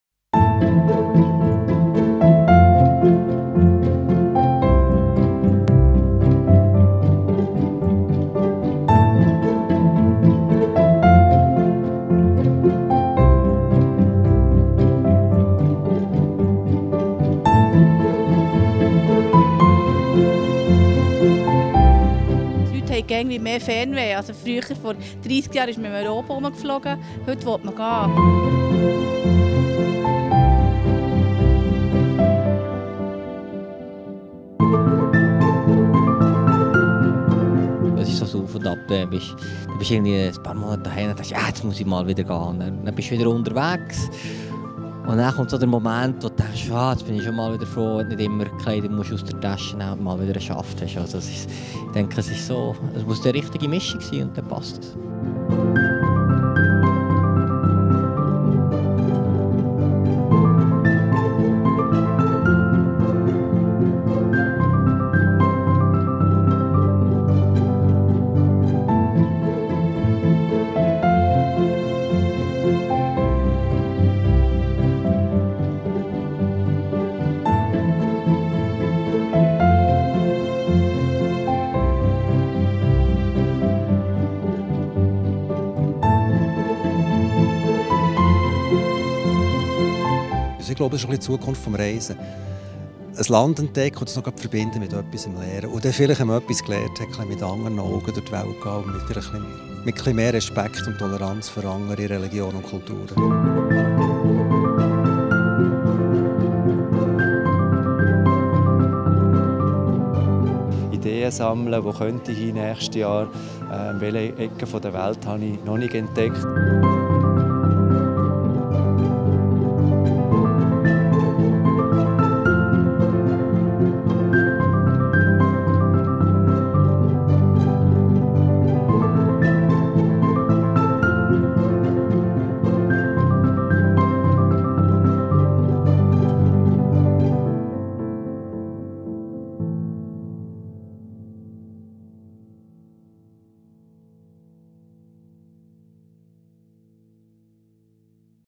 Fernwehfestival 2016: Reisefieber im Herzen von Bern
Das war das Fernwehfestival 2016. Hier haben wir uns ausgetauscht über unsere Liebe zum Reisen, unsere schönsten Erlebnisse und Bilder aus Fremden Ländern geteilt. Das nächste Fernwehfestival findet vom 27. - 29. Oktober 2017 statt.